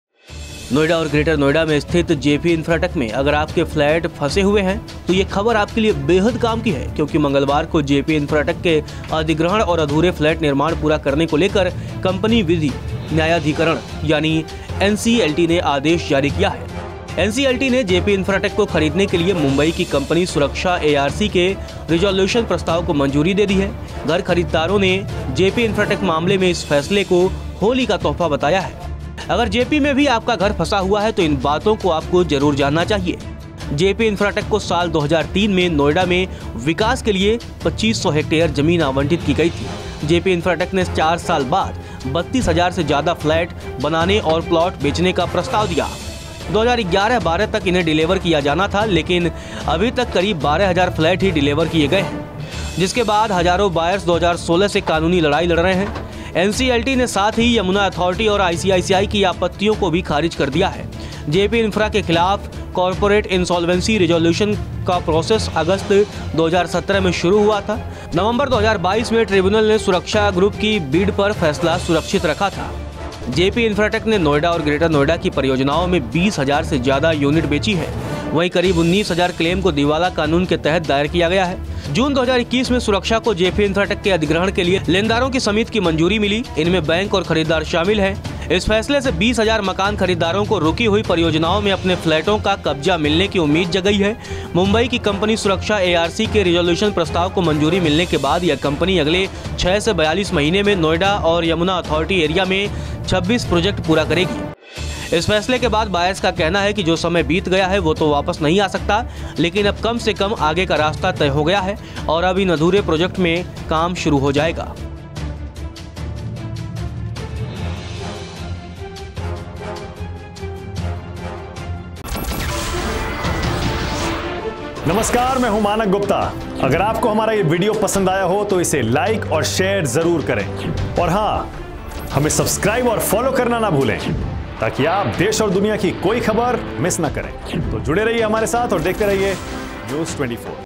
Jaypee Infratech में घर खरीदने वालों को बड़ी खुशखबरी, NCLT ने सुनाया बड़ा फैसला
नोएडा और ग्रेटर नोएडा में स्थित जेपी इंफ्राटेक में अगर आपके फ्लैट फंस हुए हैं तो ये खबर आपके लिए बेहद काम की है। क्योंकि मंगलवार को जेपी इंफ्राटेक के अधिग्रहण और अधूरे फ्लैट निर्माण पूरा करने को लेकर कंपनी विधि न्यायाधिकरण यानि NCLT ने आदेश जारी किया है।NCLT ने जेपी इंफ्राटेक को खरीदने के लिए मुंबई की कंपनी सुरक्षा एआरसी के रिजॉल्यूशन प्रस्ताव को मंजूरी दे दी है। घर खरीदरारों ने जेपी इंफ्राटेक मामले में इस फैसले को होली का तोहफा बताया है।